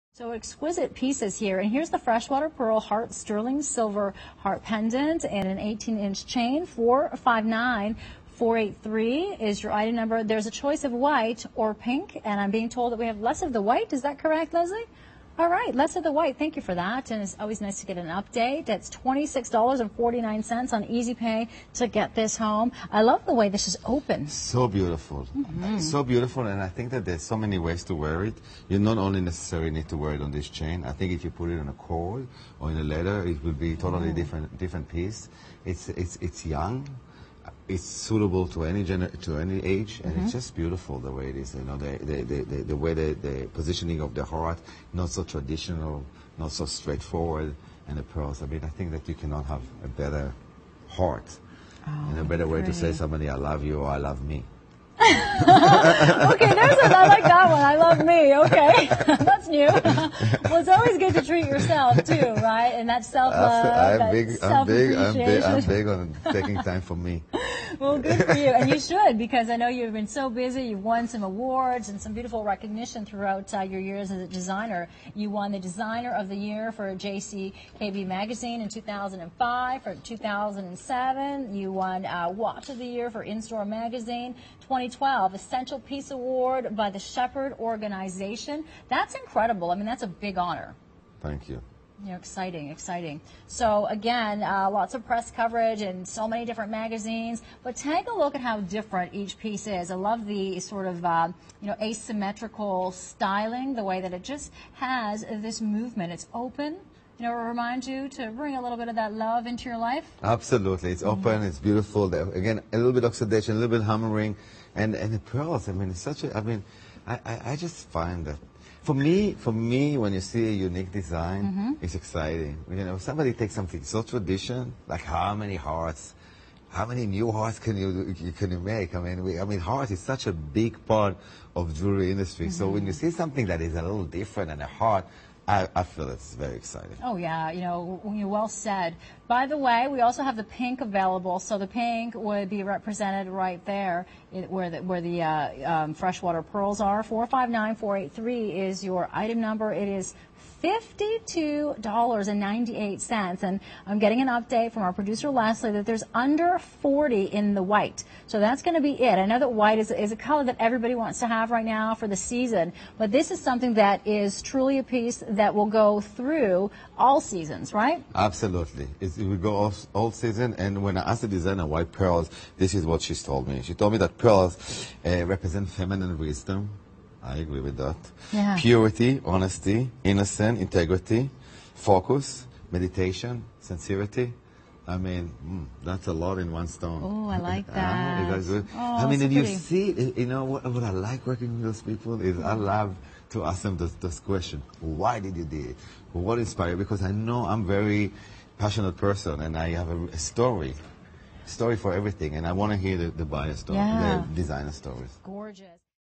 M.A.D.E. Jewellery Sterling Silver Freshwater Pearl Heart Pendant & Chain at The Shopping Channel...
Prominently displaying the heart shape, this sterling silver freshwater pearl heart pendant and chain from the M.A.D.E. Jewellery Collection makes an excellent gift for yourself or someone you love. The four pearls delicately nestled in the tilted heart pendant are what take this piece from beautiful to simply irresistible.

• Sterling silver
• (4) 4-4.5mm round freshwater pearls in pink or white
• Average gram weight is 5.90 (with pearls & chain)
• The pendant measures approximately 1 3/8" in length
• The pendant comes on a 18" chain with spring ring closure
• The pendant has an oxidized finish
• Nickel free
• Country of Origin: Israel